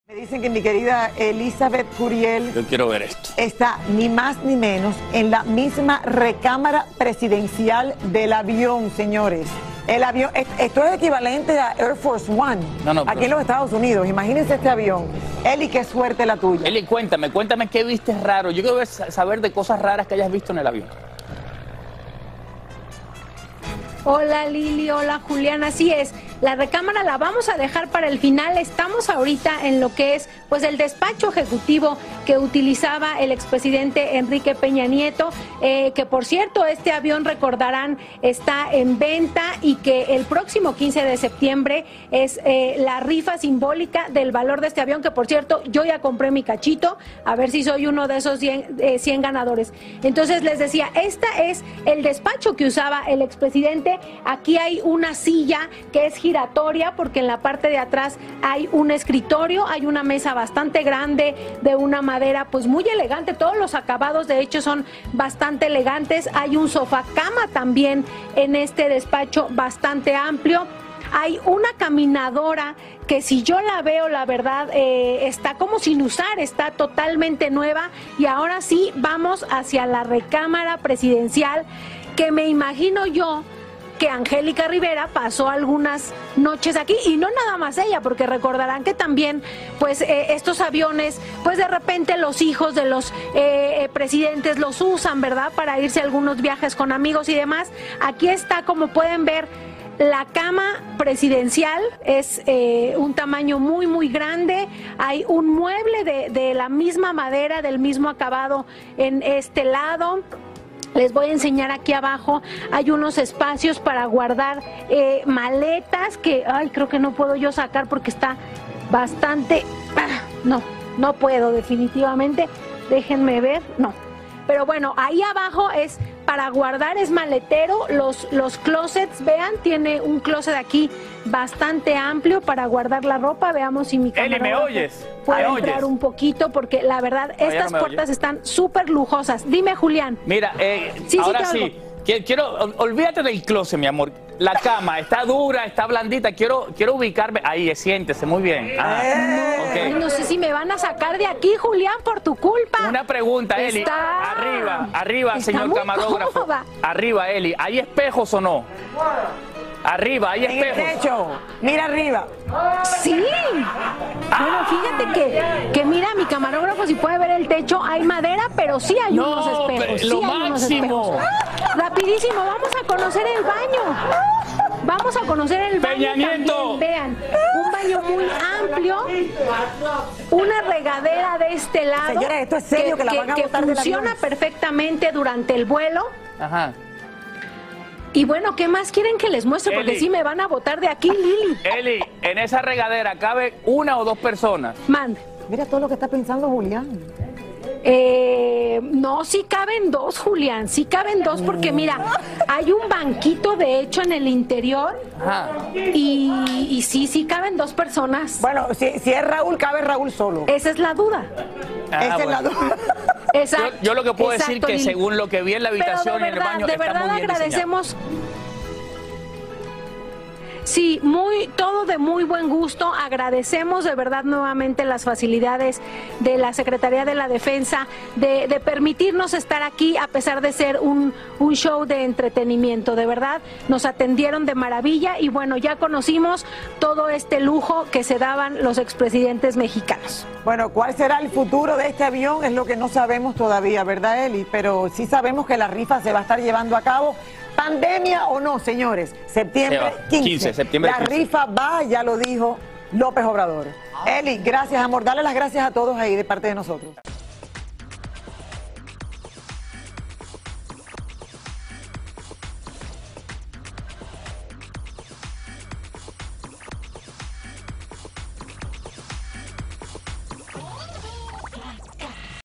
"¡Lo máximo!", dice Julián Gil al saber que la recámara del avión presidencial tiene espejos | GYF
En vivo desde el hangar presidencial de México, donde se encuentra el famoso avión que será rifado simbólicamente el próximo 15 de septiembre, nuestra reportera Elizabeth Curiel nos mostró algunos de los rincones de esta aeronave. Sin embargo, el presentador invitado se mostró muy interesado por conocer detalles de la habitación principal y esta fue su reacción cuando descubrió que sí tiene espejos en el techo.